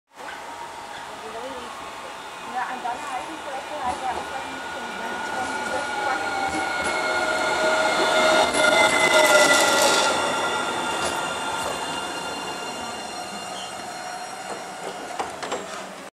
Tram 7 met reclame van used products
Tram 7 arriveert op de Crooswijksestraat. Deze tram heeft reclame van Used products.